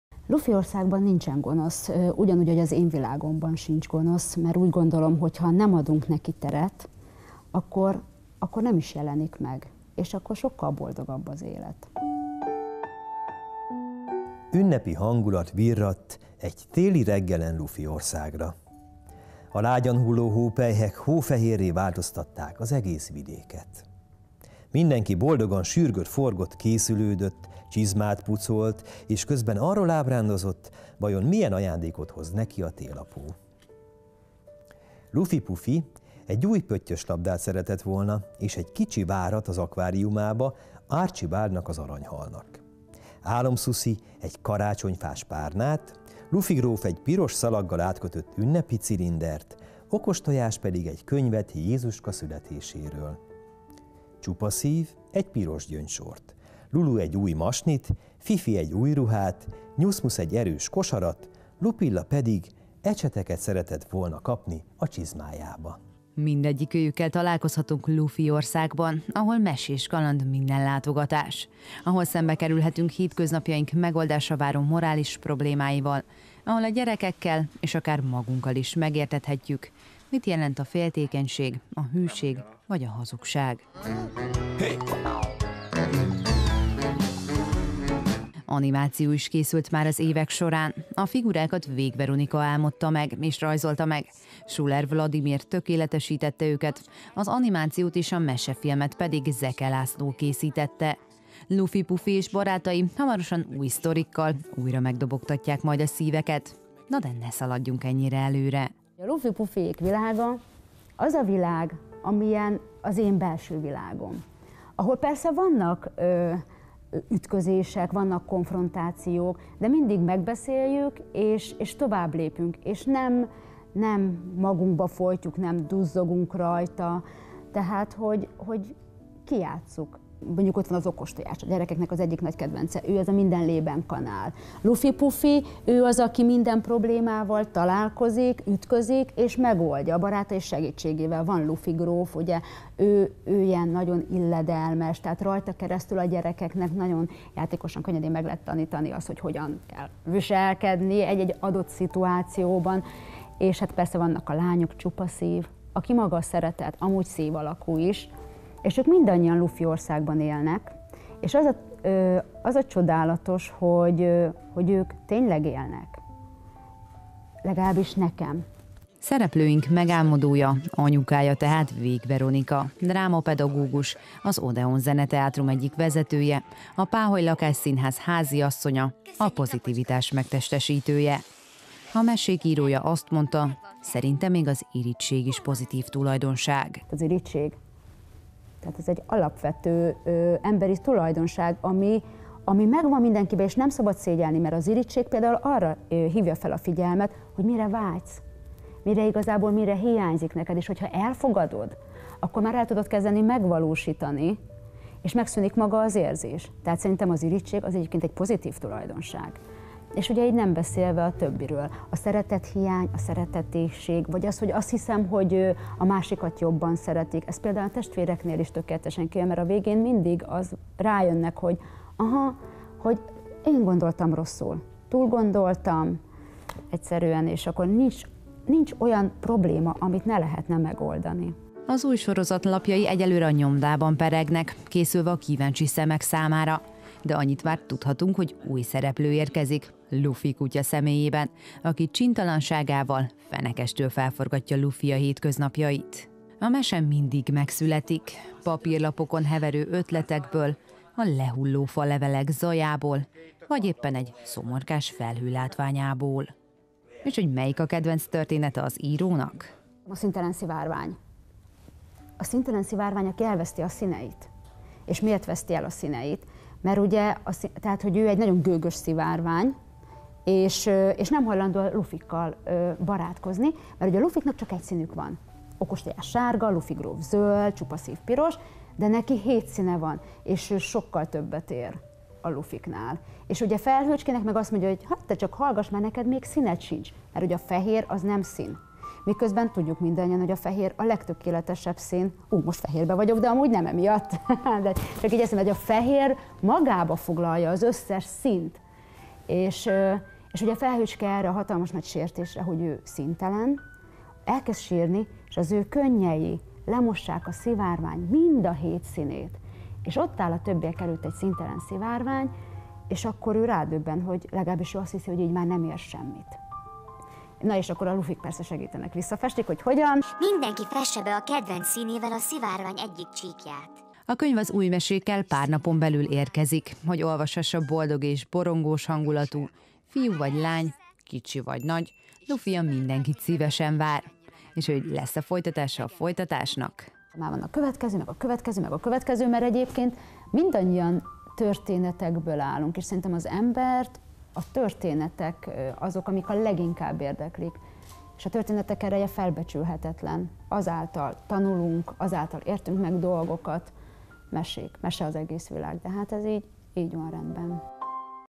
Lufipufi - Debreceni Színképek
Nyomdában 4 új Lufipufi történet. A debreceni írónő, Végh Veronika könyvei hamarosan megjelennek.